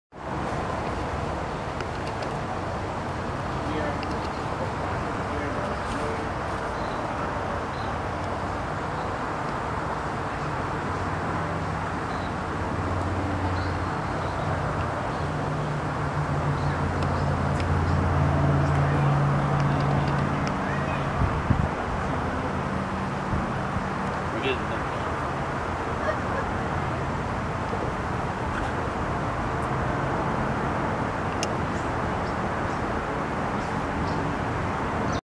Masala the red panda eating bamboo
Masala, one of the three red pandas at Zoo Knoxville, nibbles on some bamboo.